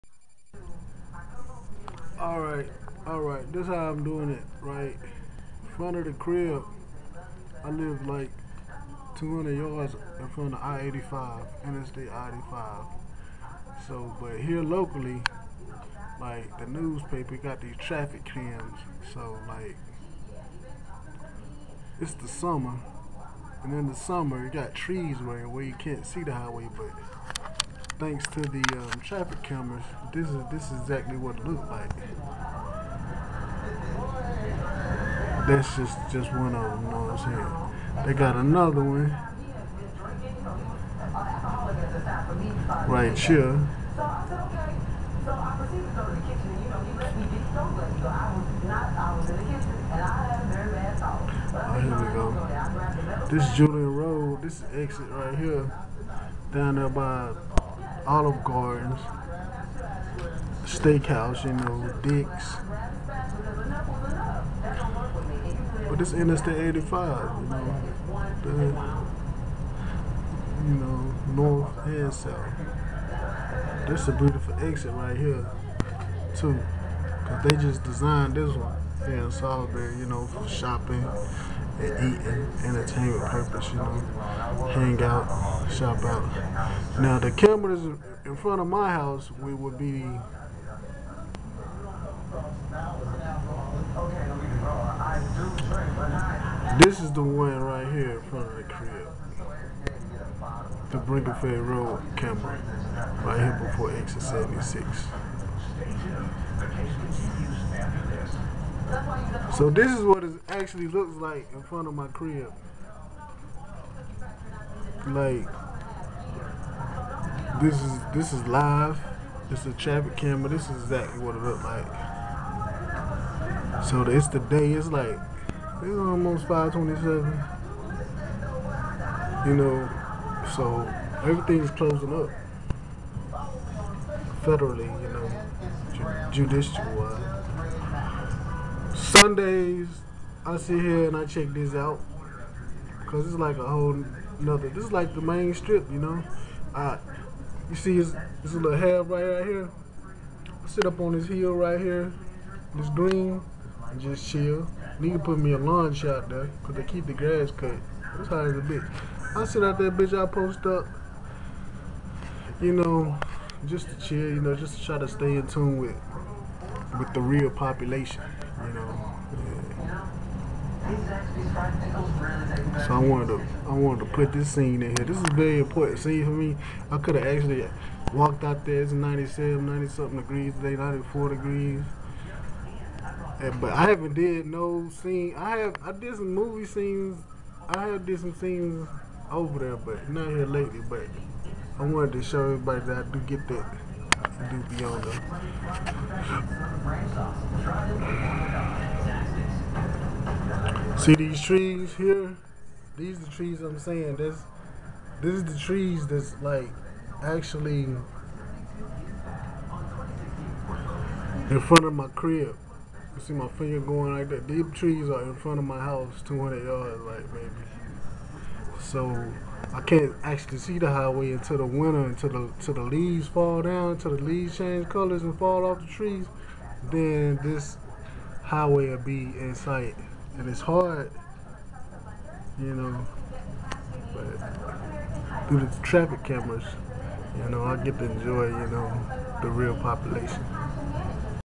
interstate 85 Salisbury/Charlotte North Carolina traffic
traffic camera too see the real population